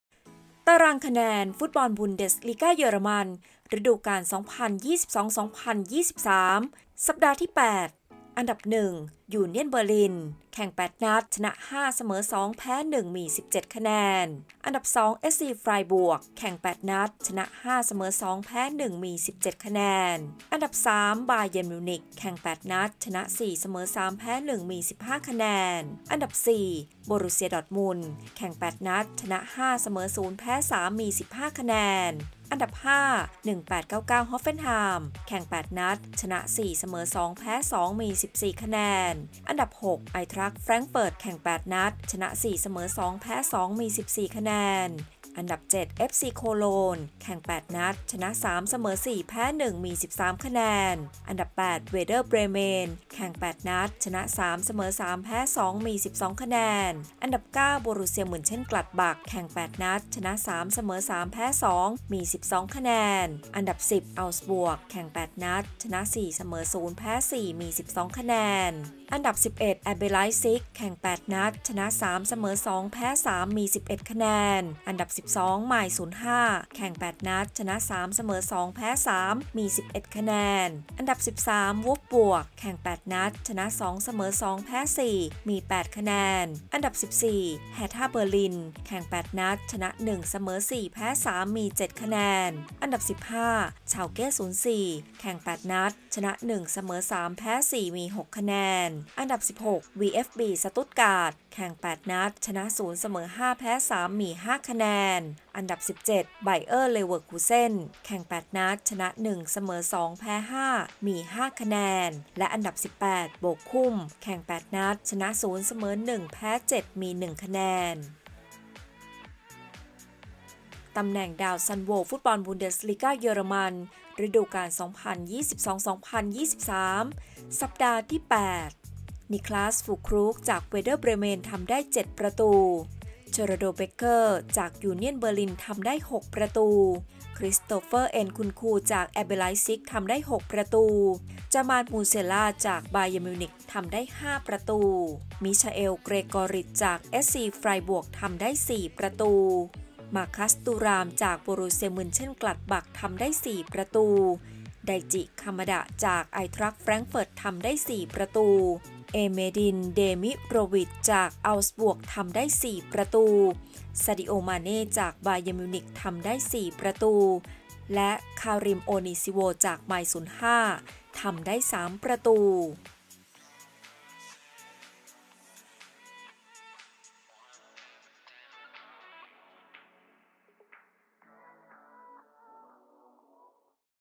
สรุปตารางคะแนน+ดาวซัลโว ฟุตบอลบุนเดสลีก้าเยอรมัน ล่าสุด (นัดที่ 8 | สัปดาห์ที่ 8)
สรุปตารางคะแนนและดาวซัลโว ฟุตบอลบุนเดสลีก้าเยอรมัน ล่าสุด (นัดที่ 8|สัปดาห์ที่ 8)
Bundesliga 2022/2023 - Standings + Goal Scorer (Week 8)

ตารางคะแนน ฟุตบอลบุนเดสลีก้า เยอรมัน
อันดับที่ 1 - ยูเนี่ยน เบอร์ลิน|| แข่ง 8 นัด || ชนะ 5 | เสมอ 2 | แพ้ 1 || มี 17 คะแนน
อันดับที่ 2 - เอสซี ไฟร์บวร์ก|| แข่ง 8 นัด || ชนะ 5 | เสมอ 2 | แพ้ 1 || มี 17 คะแนน
อันดับที่ 3 - บาเยิร์น มิวนิค|| แข่ง 8 นัด || ชนะ 4 | เสมอ 3 | แพ้ 1 || มี 15 คะแนน
อันดับที่ 4 - โบรุสเซีย ดอร์ทมุนด์|| แข่ง 8 นัด || ชนะ 5 | เสมอ 0 | แพ้ 3 || มี 15 คะแนน
อันดับที่ 5 - 1899 โฮฟเฟ่นไฮม์|| แข่ง 8 นัด || ชนะ 4 | เสมอ 2 | แพ้ 2 || มี 14 คะแนน

ตำแหน่งดาวซัลโว ฟุตบอลบุนเดสลีก้า เยอรมัน
อันดับที่ 1 - นิคลาส ฟูลครู้ก - แวร์เดอร์ เบรแมน
อันดับที่ 2 - เซอรัลโด เบกเคอร์ - ยูเนี่ยน เบอร์ลิน
อันดับที่ 3 - คริสโตเฟอร์ เอ็นคุนคู - ไลป์ซิก
อันดับที่ 4 - จามาล มูเซียล่า - บาเยิร์น มิวนิค
อันดับที่ 5 - มิซาเอล เกรกอริทช์ - เอสซี ไฟร์บวร์ก